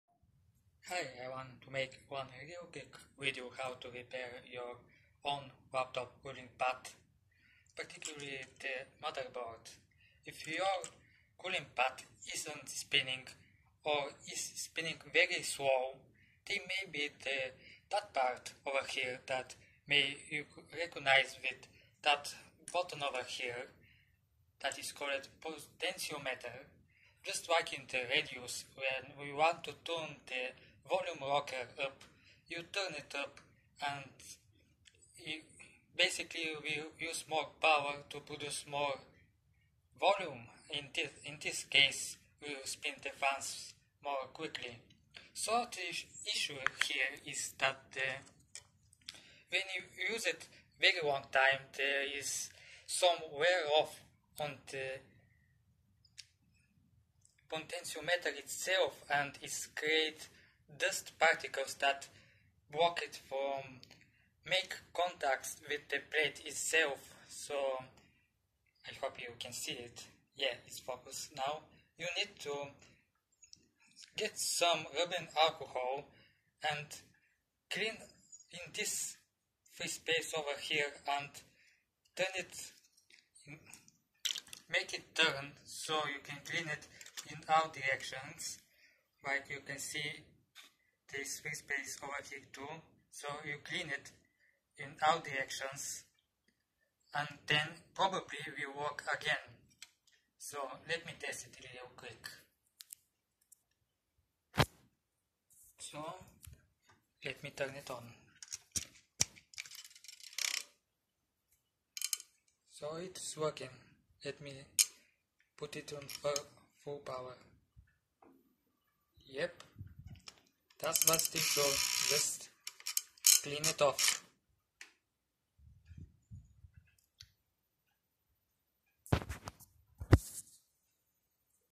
Cooling Pad fans not spinning Fix Potentiometer.
If you used for some years it eventually wear off and create dust inside the potentiometer, remove the plastic knub clean it with alcohol and will be as good as new. If that isn't the case check for corrosion crack in the solder joins, or most likely the capacitors need replacement. Good luck,.